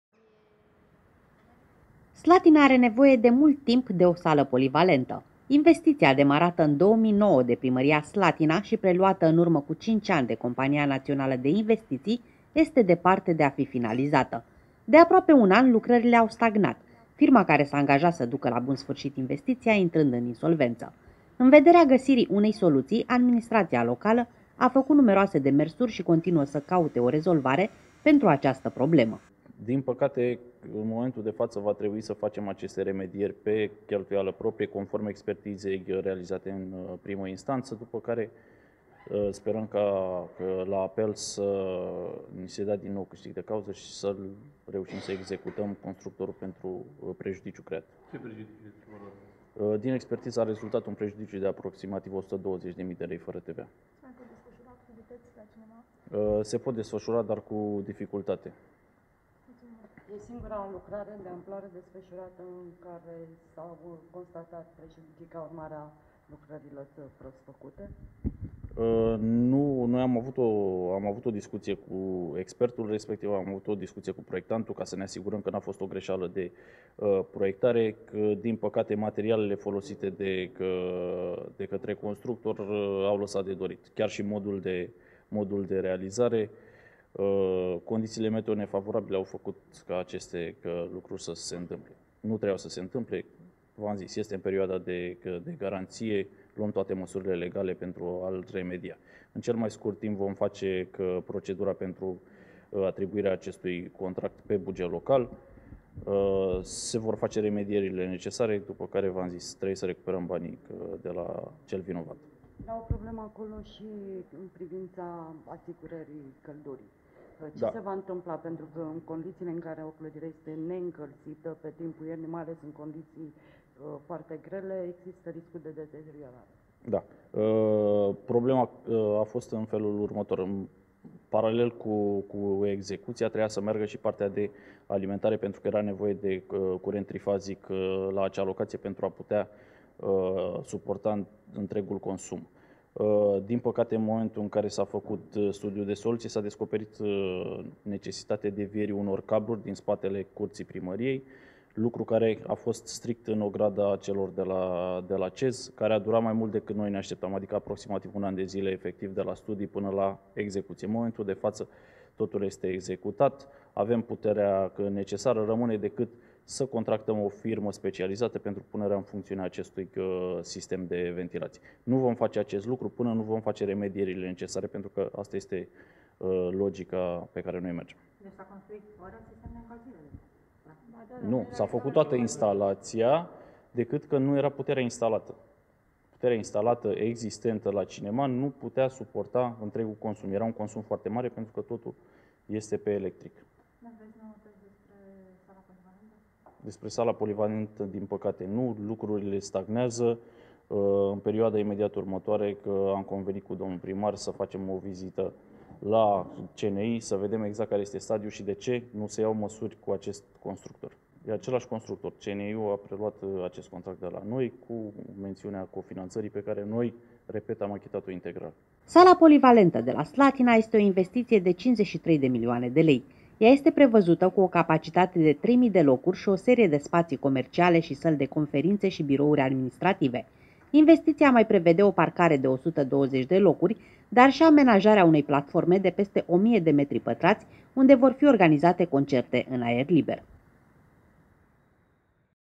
Sala Polivalentă, o investiție greu realizabilă
Nimeni nu știe când Slatina se poate mândri cu o sală polivalentă. Investiția, demarată în 2009, este departe de a fi finalizată, lucrările fiind stagnate de aproape un an. Proiectul inițiat de Primăria Slatina se află de cinci ani în custodia Companiei Naționale de Investiții. Problemele apărute sunt cauzate de intrarea în insolvență a firmei care se ocupă de construirea sălii polivalente. Primăria Slatina a făcut, în zadar, numeroase demersuri.